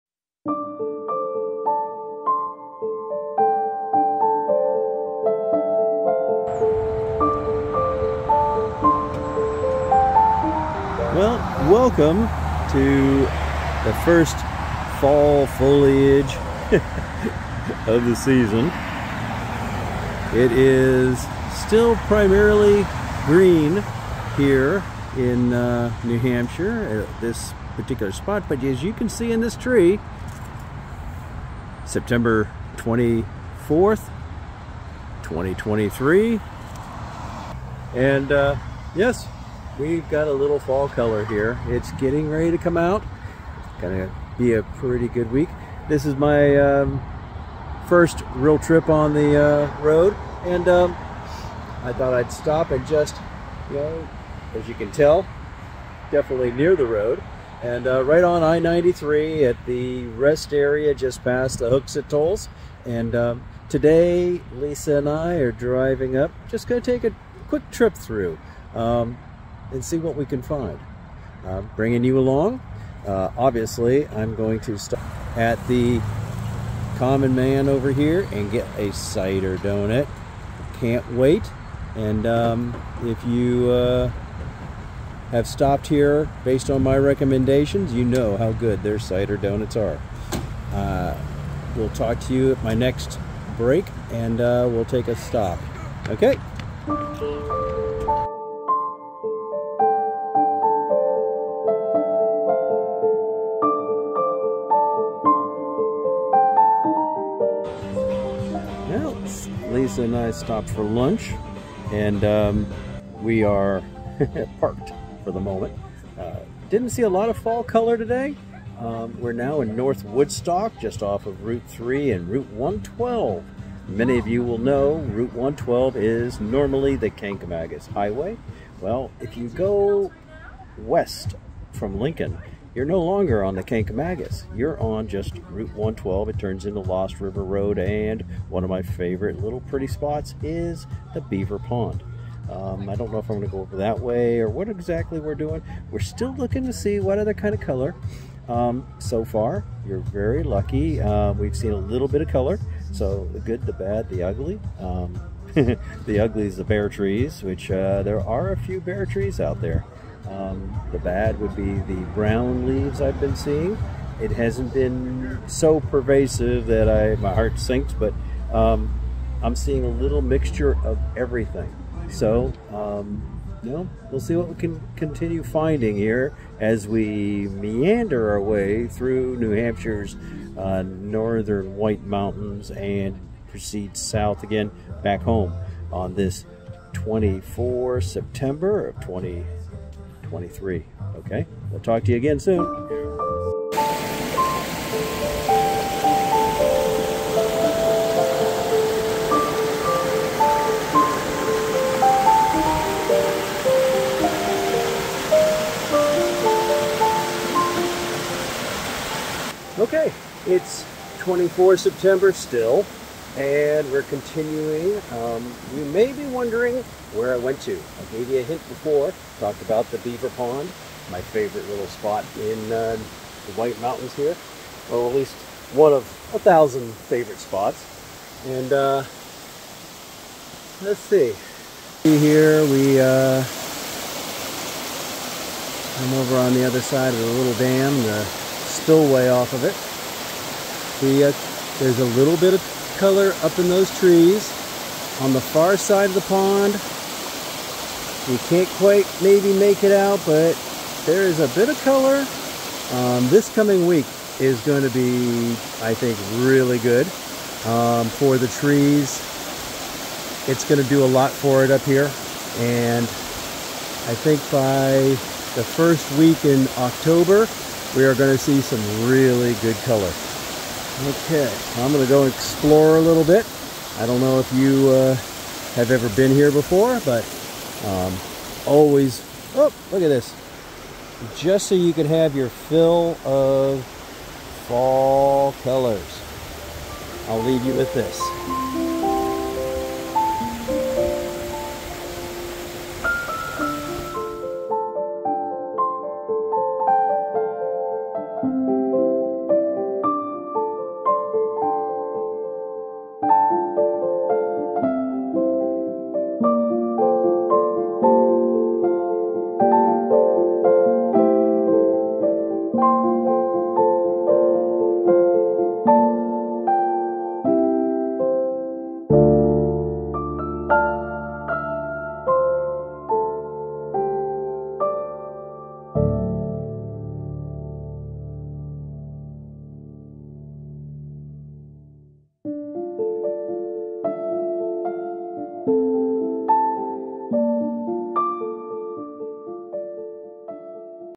Fall foliage report for 24 Sept 2023
This is my Fall foliage report for 24 Sept 2023. We traveled up I=93 to Waterville Valley, The Beaver Pond along Route 112, had lunch in Woodstock NH, and then came back over to I-93 and headed home.
We did not find really great fall colors but with the sun last week and the partly cloudy to sunny conditions this coming week I think the 1st of October should be pretty nice.